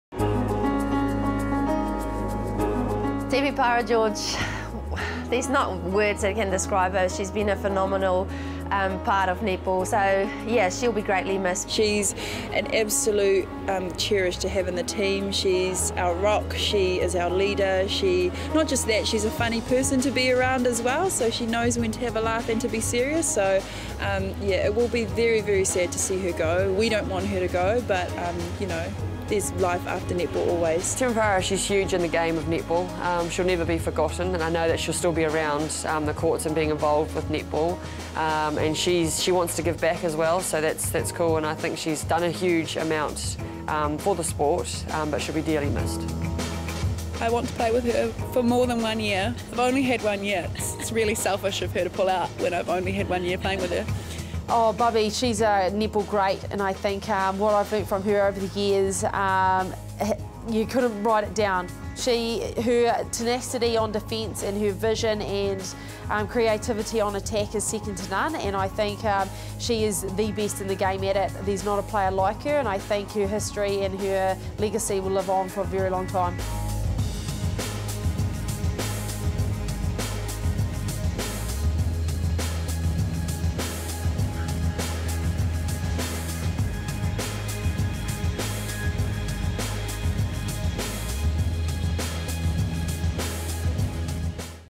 Temepara George Tribute
A tribute to Temepara George. George played in every game the Mystics played in the ANZ Championship and her teammates talk about how much she will be missed.